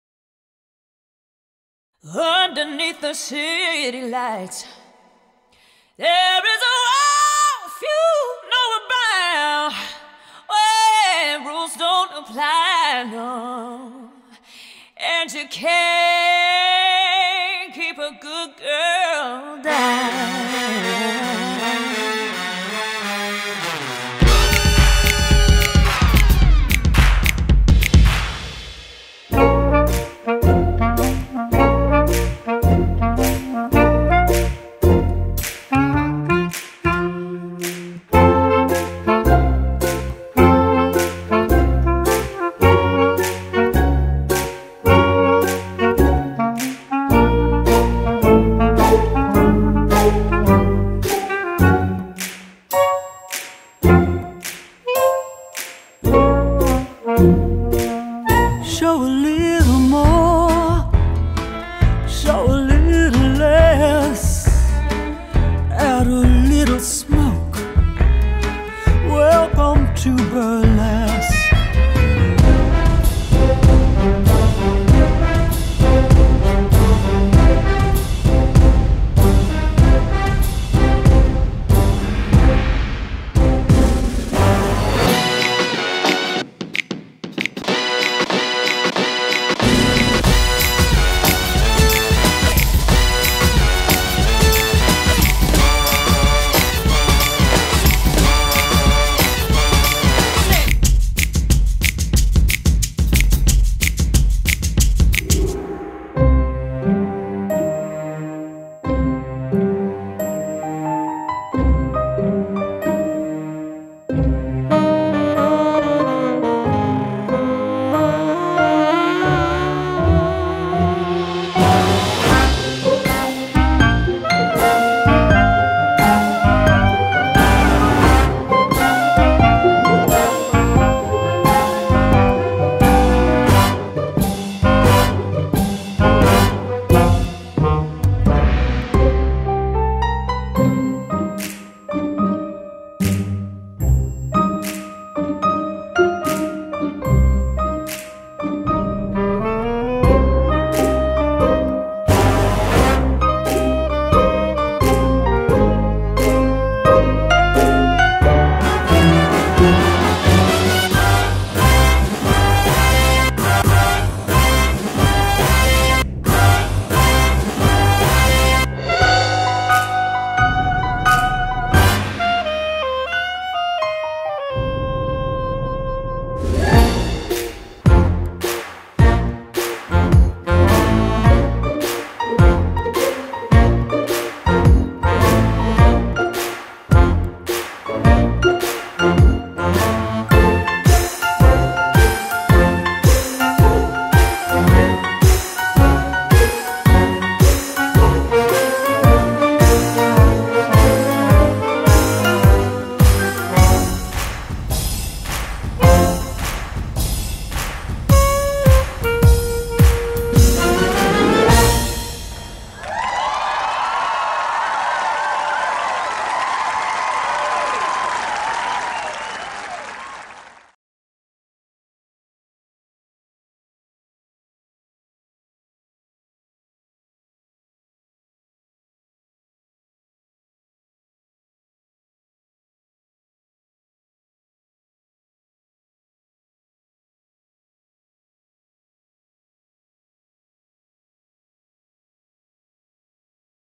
Burlesque Dressage Freestyle - My Lady - Mikala Munter
Celebrating our final Burlesque Grand Prix Dressage Freestyle of "My Lady".  Owned by Janne Rumbough, ridden by Mikala Munter with music arranged by Gary Patterson of Patterson Dressage Music. Enjoy...

Author makes no claim to ownership of the original musical works featured in the video.